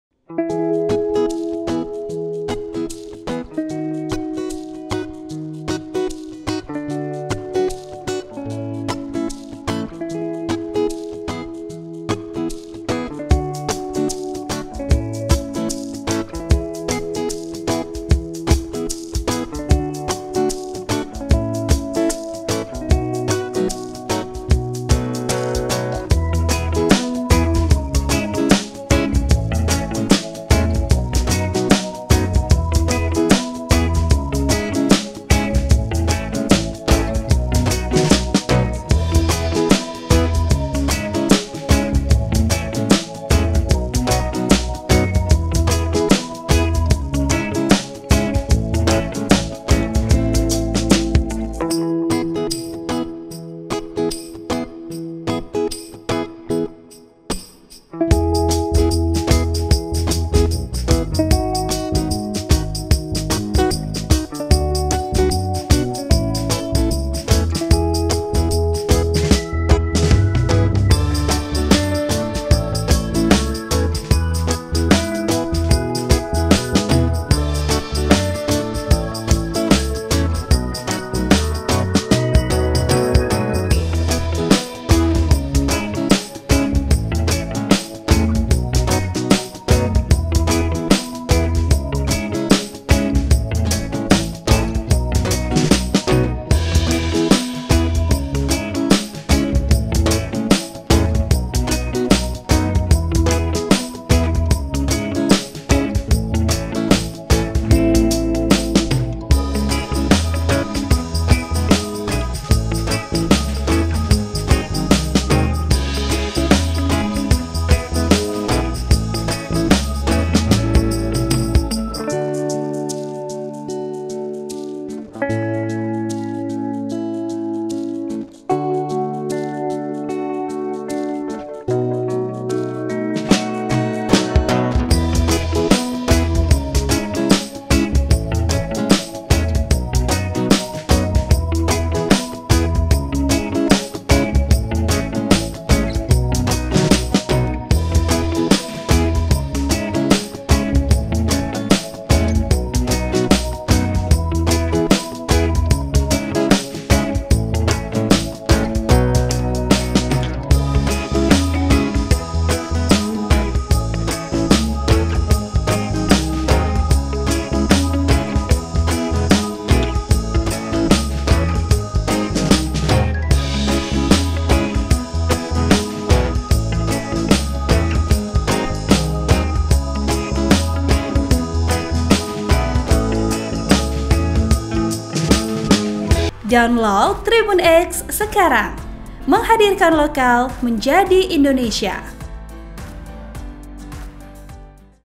276.801 Wisatawan Kunjungi Taman Nasional Komodo Manggarai Barat, Wisman Mendominasi
TRIBUNFLORES.COM, LABUAN BAJO - Sebanyak 276.801 wisatawan tercatat mengunjungi Kawasan Taman Nasional (TN) Komodo, Labuan Bajo, Manggarai Barat, Nusa Tenggara Timur sepanjang Januari-November 2023.

Kepala Balai Taman Nasional Komodo Hendrikus Rani Siga, menyebut jumlah kunjungan itu didominasi wisatawan mancanegara (wisman).

"Rinciannya wisatawan mancanegara (wisman) 172.914 orang dan wisatawan domestik 103.887 orang," jelas Hendrikus, Selasa 19 Desember 2023.

Artikel ini telah tayang di Pos-Kupang.com dengan judul 276.801 Turis Kunjungi Taman Nasional Komodo Manggarai Barat, Wisman Mendominasi,

Penulis: Engelbertus Aprianus
Editor: Oby Lewanmeru
VP: Ian Taopan